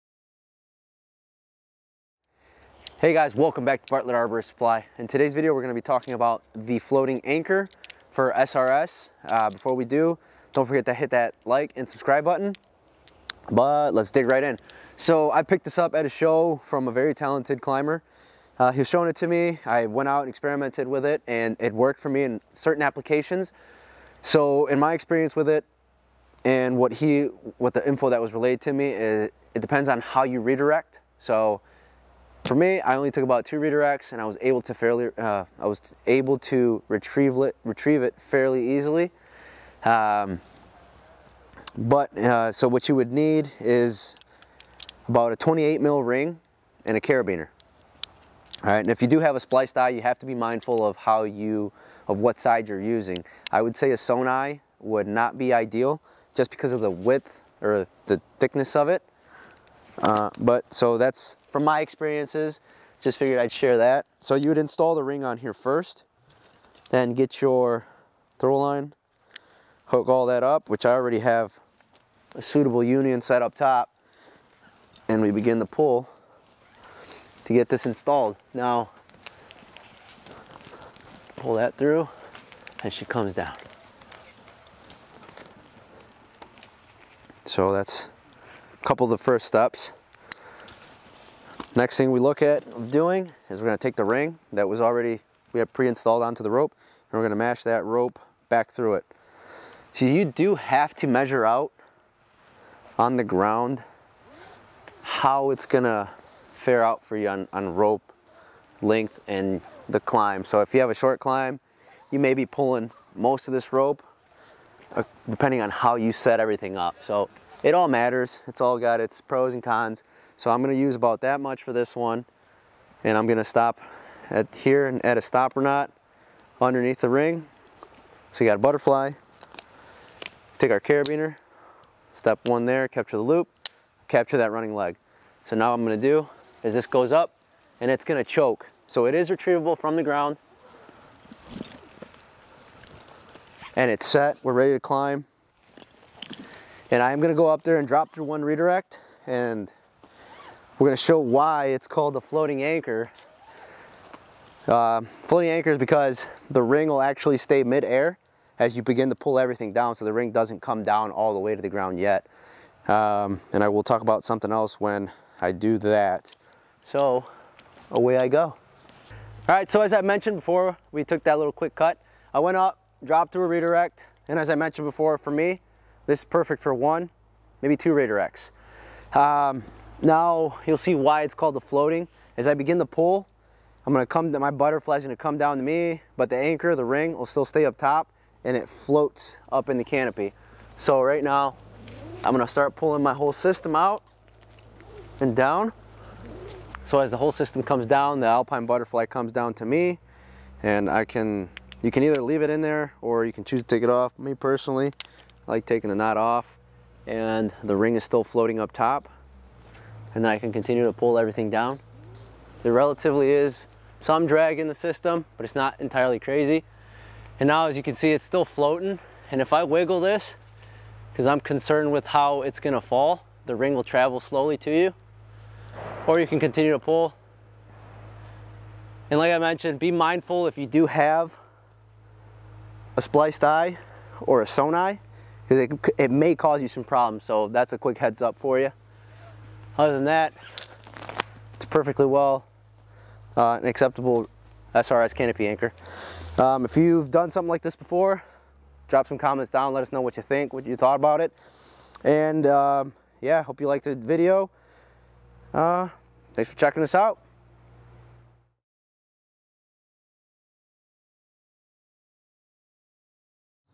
SRT/SRS "Floating Anchor"
The Floating Anchor, is a quick and easy canopy anchor to employ!